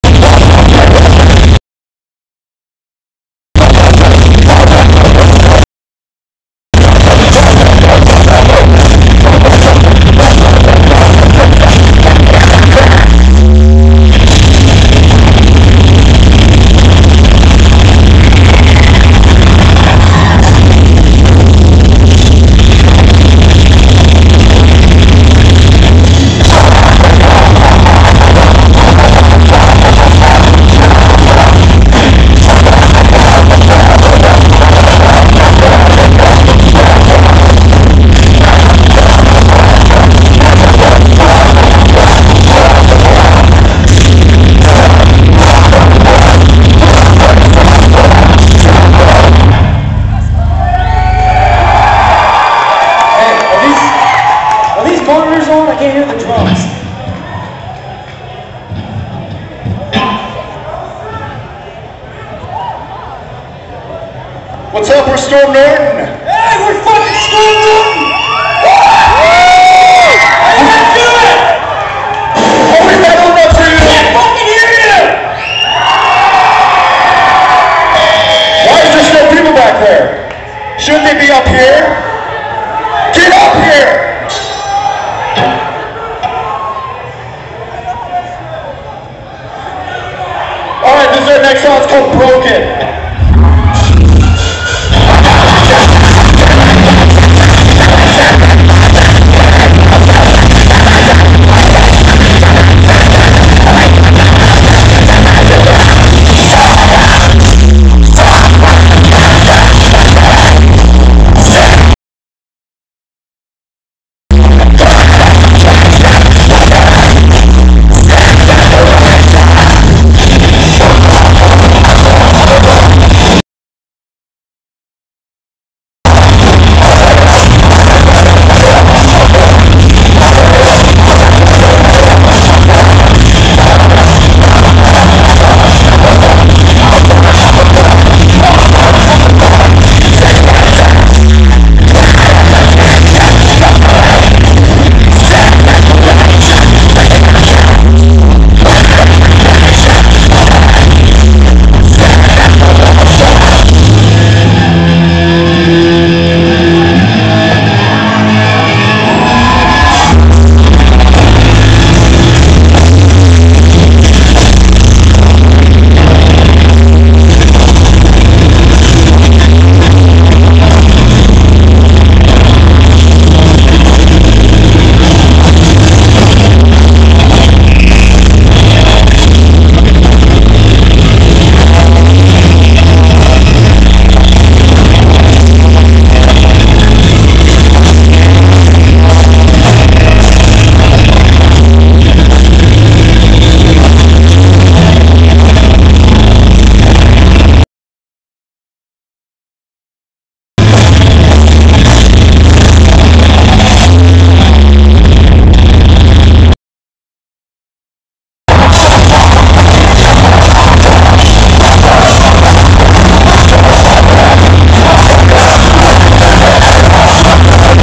We are STORM NORTON!
WARNING: turn down sound! Awesome job Brad, Bryce, Braden, Edd, and Dillon! YOU SMOKED THOSE OTHER BANDS! Filmed on location at the Next Big Thing Tour on the main stage at the Roseland Theater on Sat. Feb. 20th, 2010! Cuts out a little here and there and audio crazy but an fn sick video! YOU ARE THE NEXT BIG THING!